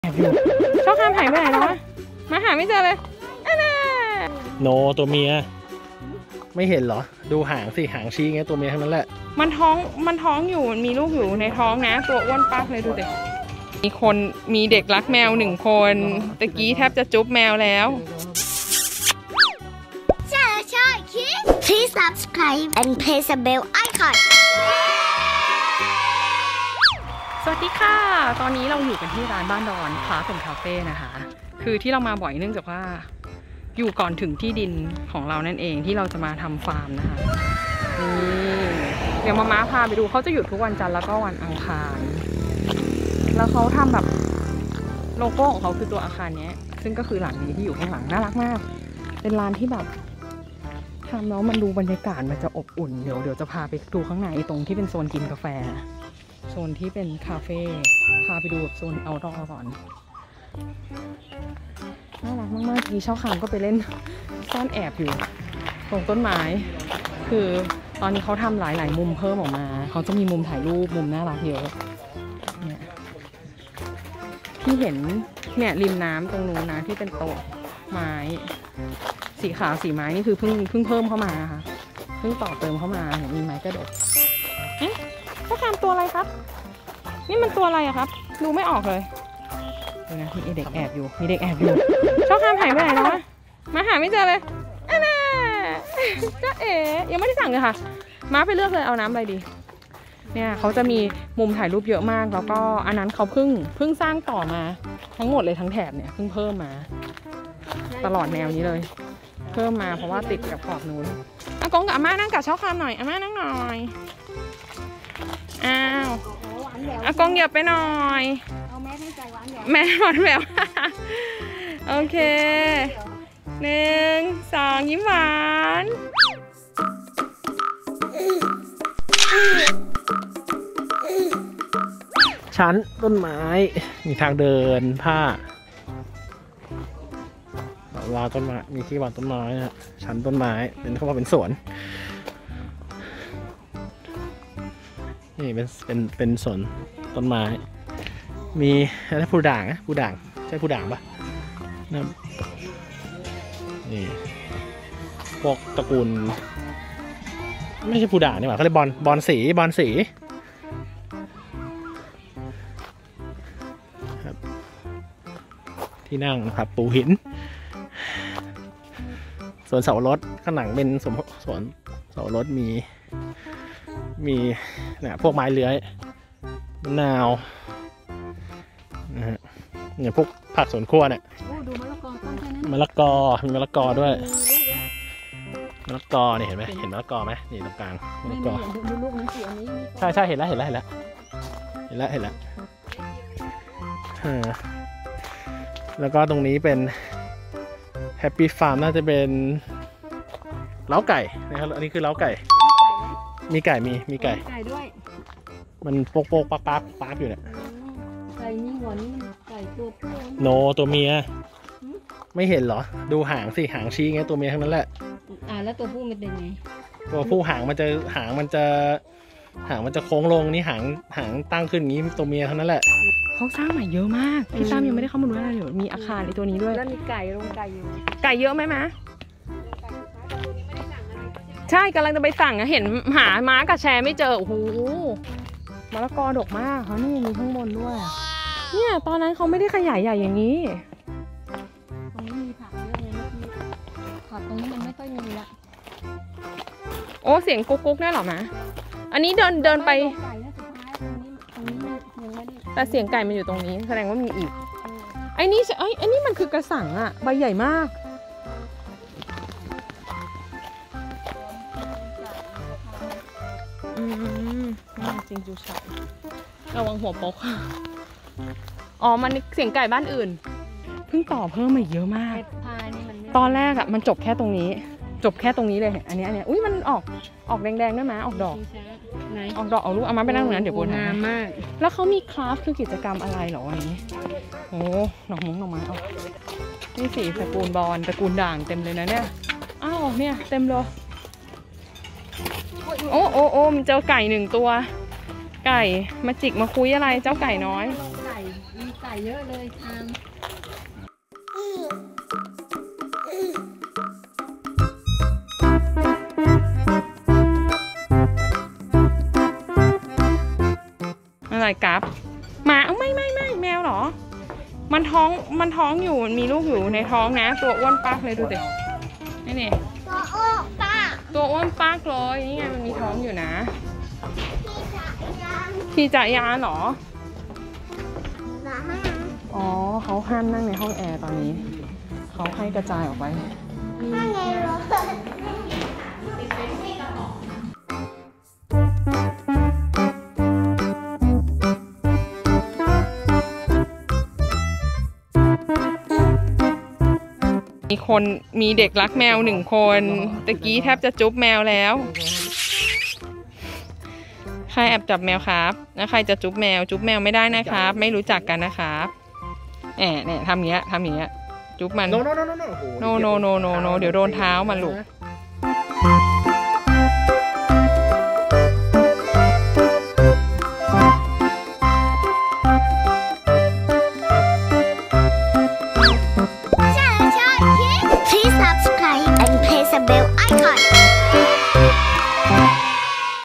ช่อช่อคิดส์ | พักผ่อนหย่อนใจที่บ้านดอน คราฟท์ คาเฟ่ Baandon craft x cafe
อากง อาท่า พาเที่ยว บ้านดอน คราฟท์ คาเฟ่ Baandon craft x cafe
จ.ปทุมธานี